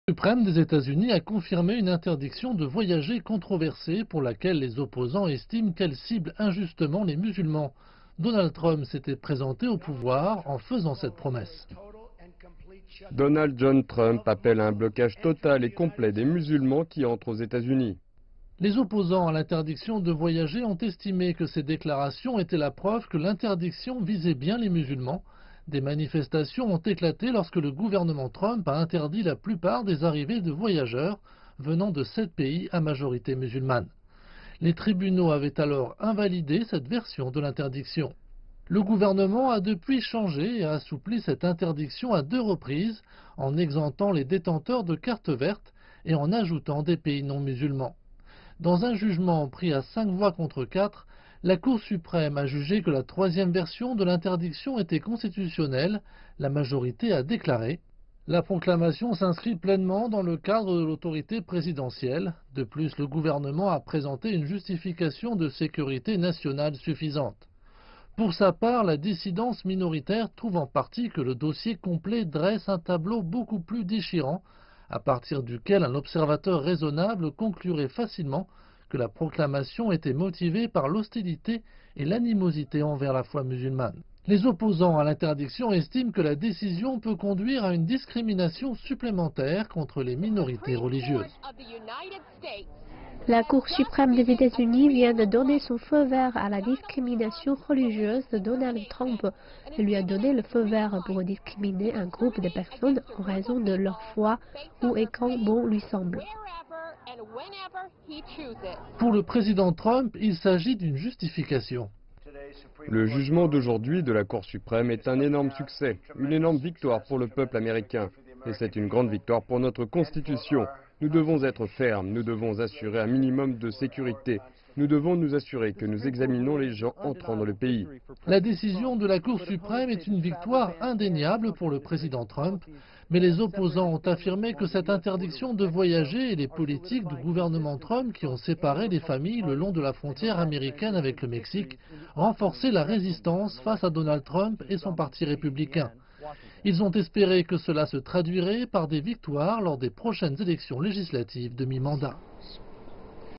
Contrôle migratoire validé
La Cour suprême des États-Unis a confirmé l'interdiction de voyager controversée du président Donald Trump. Cette interdiction touche plusieurs pays principalement des pays musulmans. Dans une décision prise à 5 voix contre 4, la haute cour n'a pas établi que la politique était discriminatoire à l'encontre des musulmans.